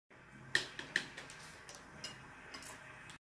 Kimmy 16th Birthday Dinner
Kimmy 16th Birthday. Hot pot dinner with Grandpa.  Kobe beef, shrimp,  squid, fish, and lots of vegetables. Dinner with Daddy and Phuong. March 20, 2020.